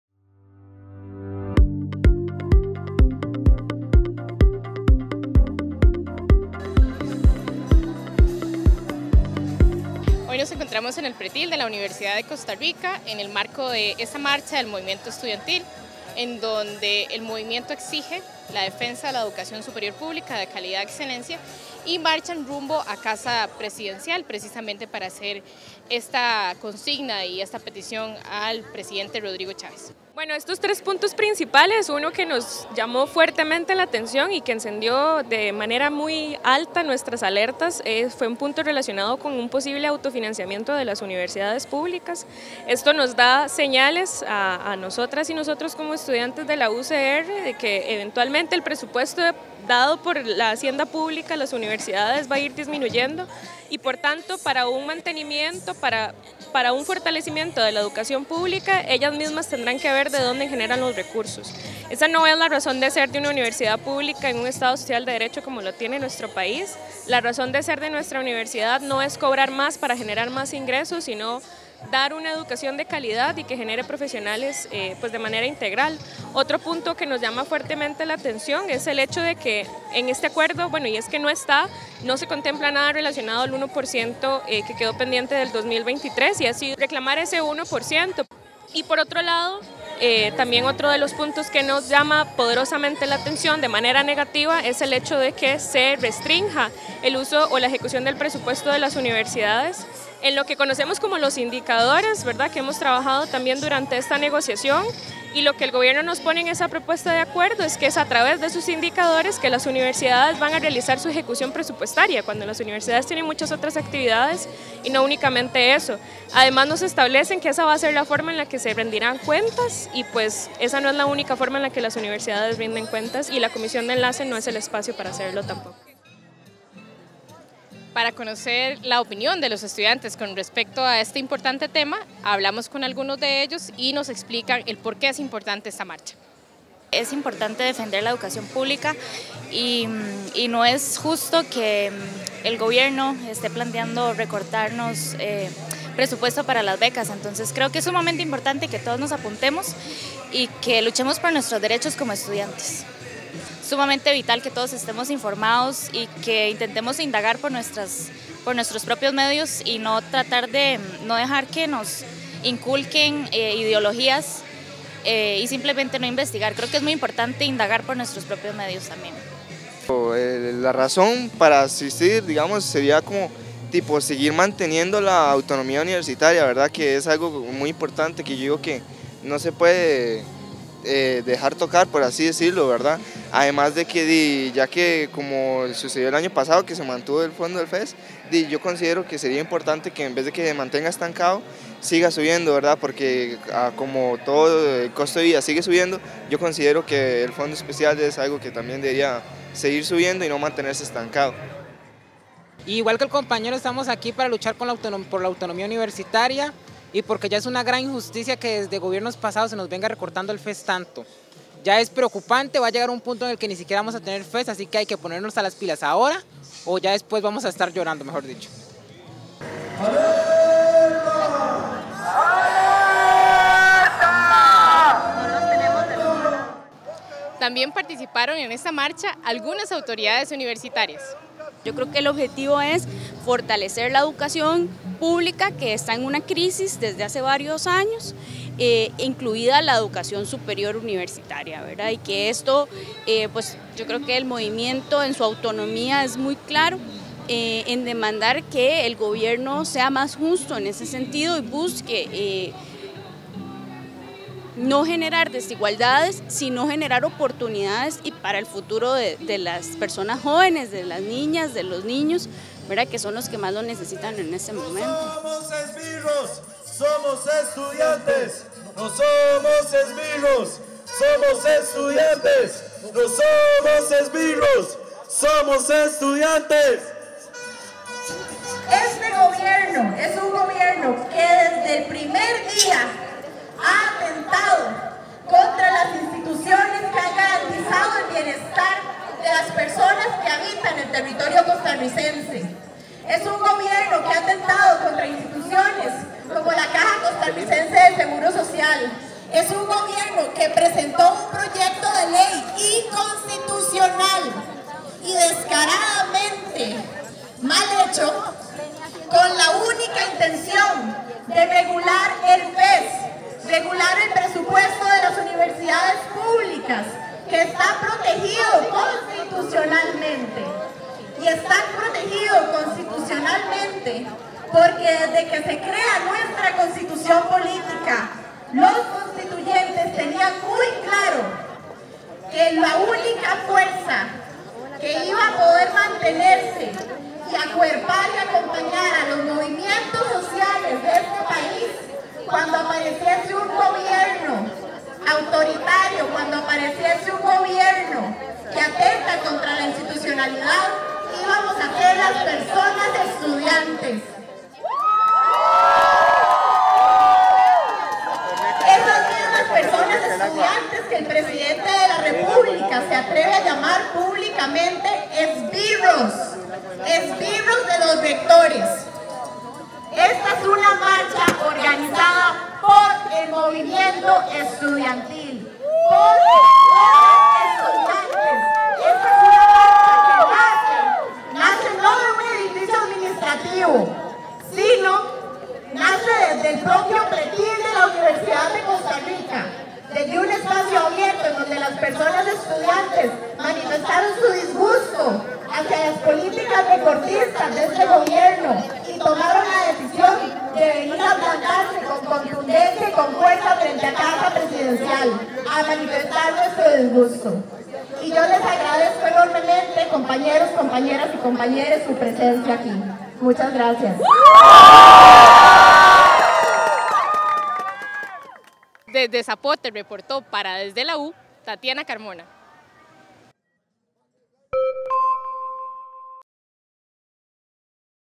Desde la U: Marcha en defensa del FEES
Desde la U nos presenta las voces de la marcha en defensa del Fondo Especial para la Educación Superior (FEES), la cual se realizó este martes 22 de agosto convocada por la Federación de Estudiantes de la Universidad de Costa Rica.